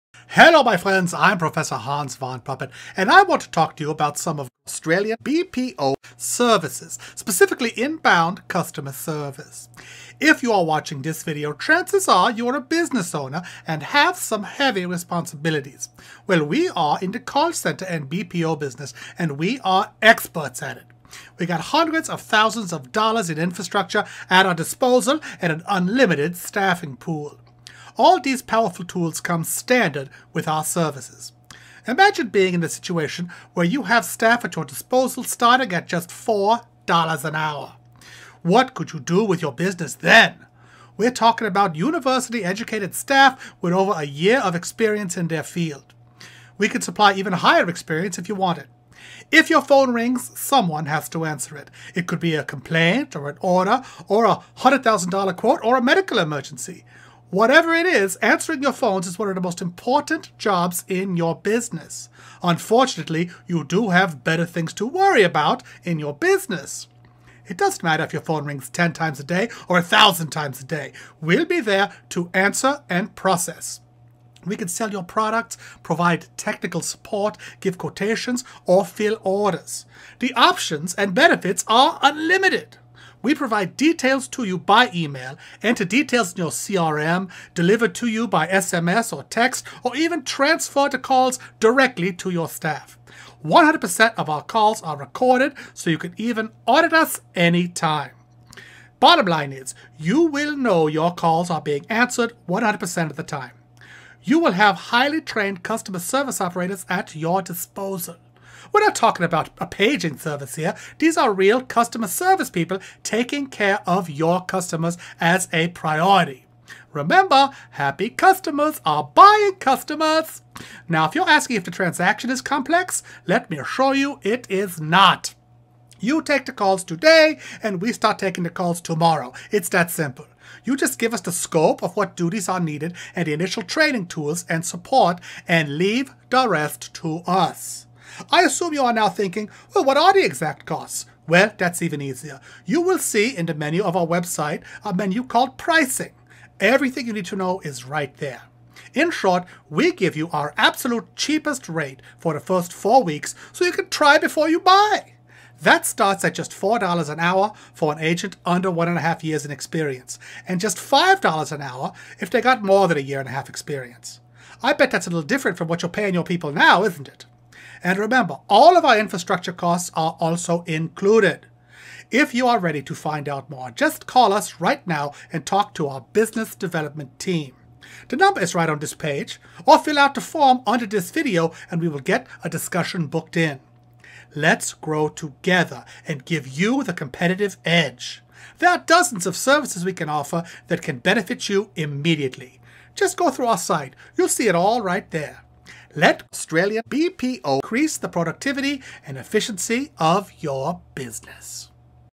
Inbound Customer Service of AustraliaBPO
Hi my name is xxxxxxxx and I want to talk to you about some of AustraliaBPO Solutions services, specifically Inbound customer service.

If you are watching this video, chances are you are a business owner and have heavy responsibilities including Customer Service.

We are in the Call Center and BPO business and are experts at it. We have hundreds of thousands dollars in infrastructure at our disposal and an unlimited staffing pool. All these powerful tools come standard with our Customer Service.

Imagine being in the situation where you have staff at your disposal starting at $4 per hour. What could you do to your business then? We are talking about university educated staff with over 1 year experience in their field,We can supply even higher experience if you require.

If your phone rings, someone has to answer it. It could be a complaint, an order, $100,000 quote or a medical emergency. Either way, answering your phones is one of the most important jobs in your business. Unfortunately you do have better things to worry about in your business.

It doesn’t matter if your phone rings 10 times a day or 1000 times a day, we will be there to answer and process. We can sell your products, provide technical support, give a quotation, fill an order. The options and benefits are unlimited.

We provide details to you by email, enter details in your crm, deliver to you by sms or txt, or even transfer it directly to you or your staff. 100% of our calls are recorded so you can even audit us anytime.

Bottom line is, you will know your calls will be answered 100% of the time. You will have highly trained customer service operators at your disposal. We are not talking about a paging service here, but real customer service taking care of your customers as a priority. Remember happy customers are buying customers.

Now if you are asking if the transaction is complex, let me assure you it is not. You take your calls today and we take the calls tomorrow. It’s that simple. You just give us the scope of what duties are needed and the initial training tools and support and leave the rest to us.

I assume you are now thinking what are the exact costs, well that is even easier. You will see in the menu on our web site called “Pricing”, everything you need to know is there.

In short we give you our absolute cheapest rate for the 1st 4 weeks so you can try before you buy. That starts at $4 per hour for an agent under 1 ½ years’ experience and $5 per hour for over 1 ½ years’ experience above. I bet that is a bit different from your salary costs you are paying now. And remember all of our infrastructure costs are also included.

The number is right on this page OR fill in the form under this video and we will get a discussion booked in.

Let’s grow together and give you the competitive edge. There are dozens of services we offer that can benefit you immediately, just go through our site, it is all there.

Let AustraliaBPO help you increase the productivity and efficiency in your Business.